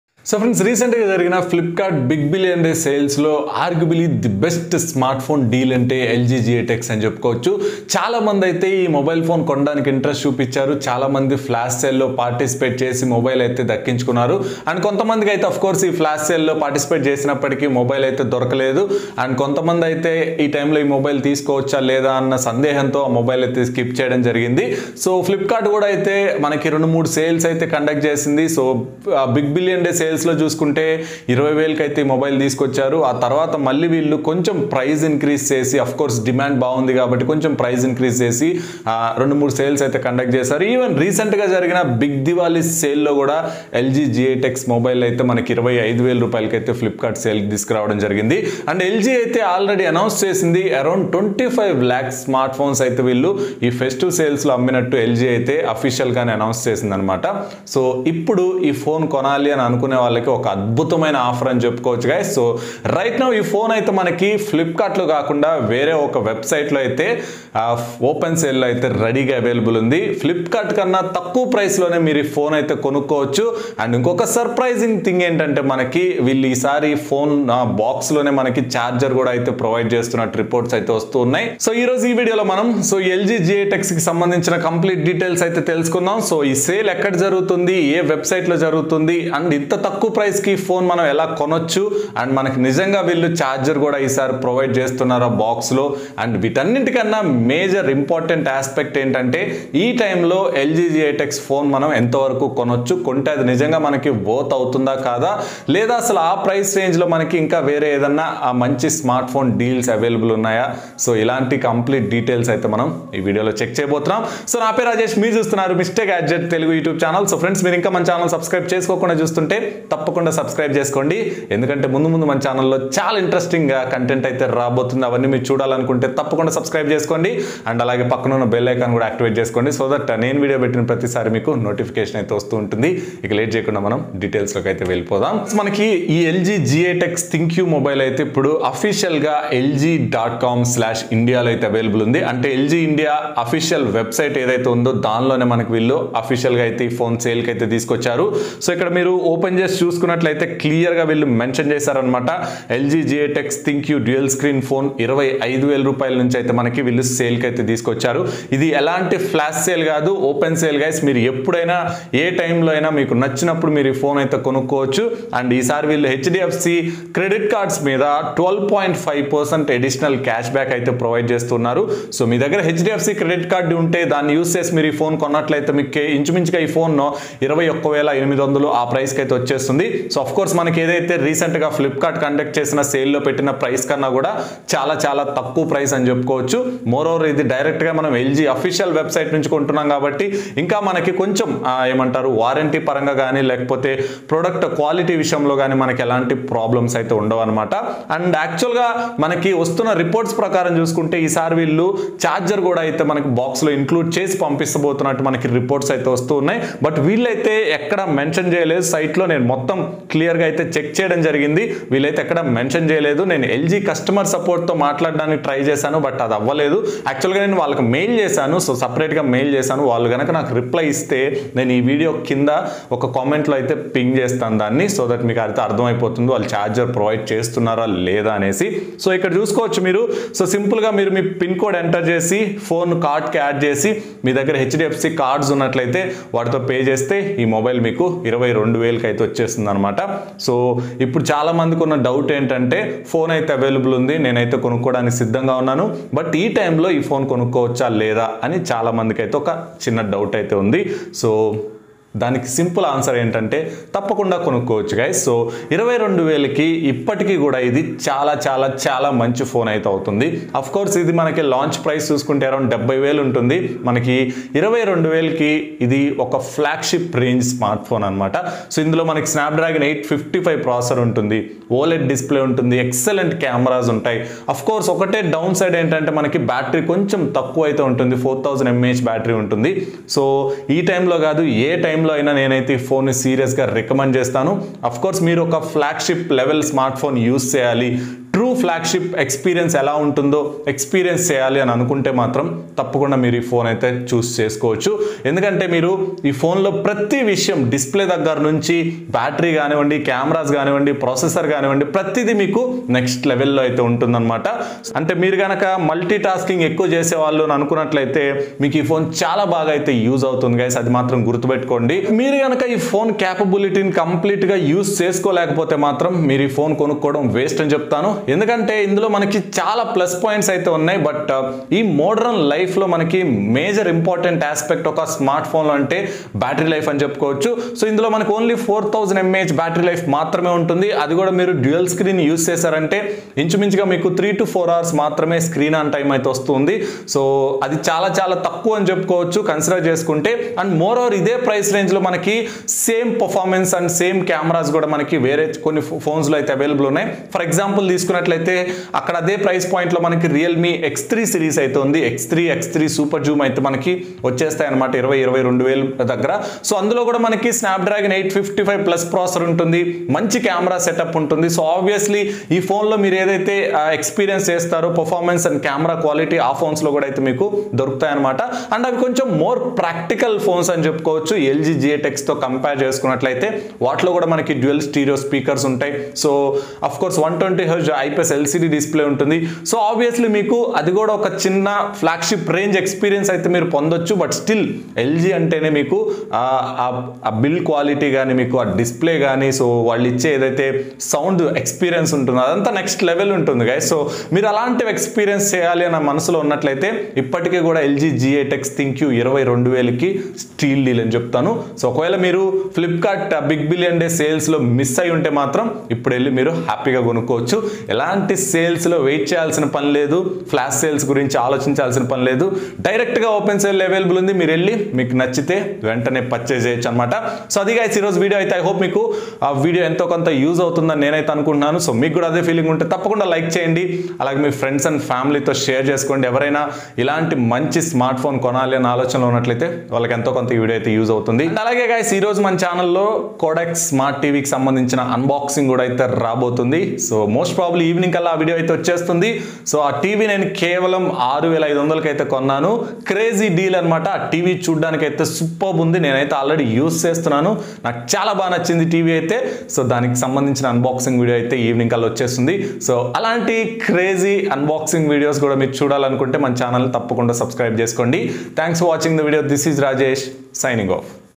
LG G8x Available in Open Sale at an Unbelievable Price | This time with Charger in Box ...?
LG G8x ThinQ is now available in open sale at a much lower price than Flipkart. Grab it before it gone out of stocks.
If you find this video helpful, please give it a big Thumbs up.
and to do more awesome videos for you.